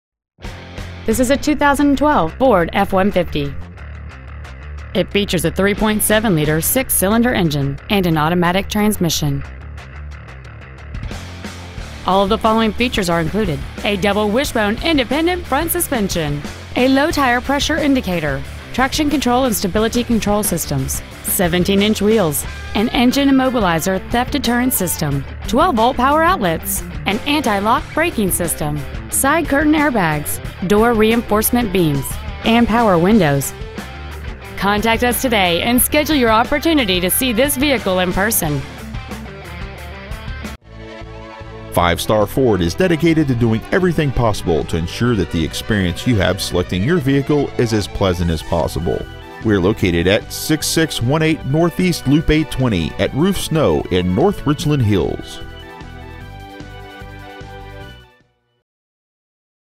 2012 Ford F-150 North Richland Hills TX
Year : 2012
 Make : Ford
 Model : F-150
 Engine : 3.7L V6 24V MPFI DOHC FLEXIBLE FUEL
 Trans . : 6-SPEED AUTOMATIC
 Exterior : PALE ADOBE METALLIC

 Stock : CFA40169